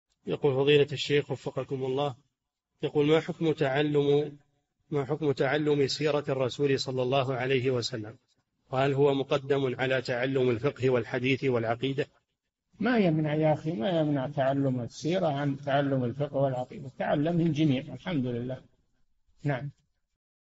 [4223- 4717] هل يقدم تعلم السيرة على الفقه والعقيدة والحديث؟ - الشيخ صالح الفوزان
"[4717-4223] ما حكم تعلم سيرة الرسول ﷺ؟ وهل هو مقدم على تعلم الفقه والحديث العقيدة؟
الجَوَابُ: ما يمنع يا أخي، ما يمنع تعلم السيرة عن تعلم الفقه والعقيدة تعلّم الجميع والحمد لله!

للاشتراك في قناة الشيخ صالح الفوزان
فتاوى المجالس في تفسير المفصل

للاستفسار والإقتراحات والدعم
رئيس الفريق
لمشروع كبار العلماء